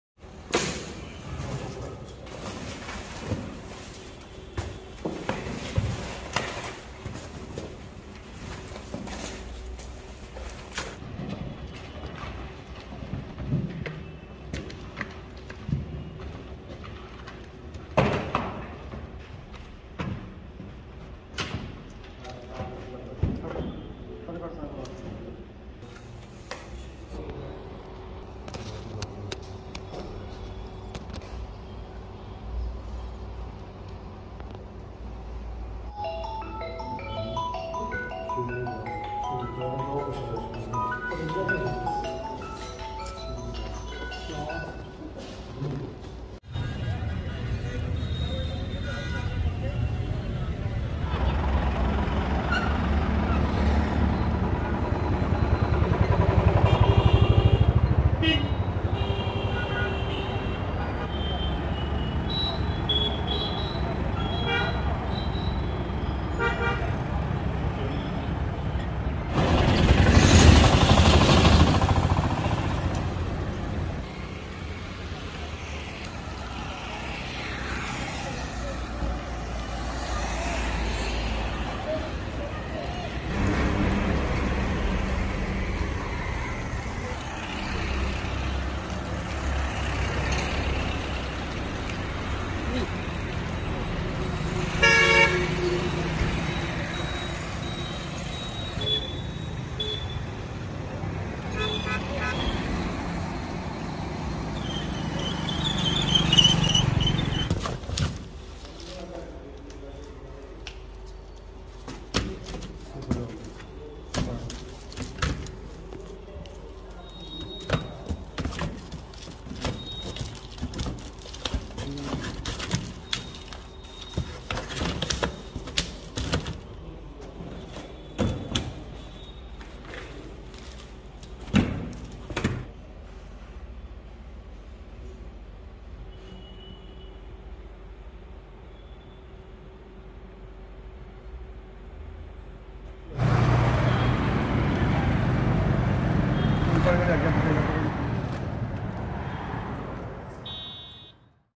पुलिस ने पकडे '500 के करीब चायनीज़ मांझा के रोल
Good Job Team Kathua Police
Lakhanpur। पुलिस ने पकड़ी 'मौत की डोर': 4 कार्टून माल बरामद, 500 के करीब चाइनीज मांझे के थे रोल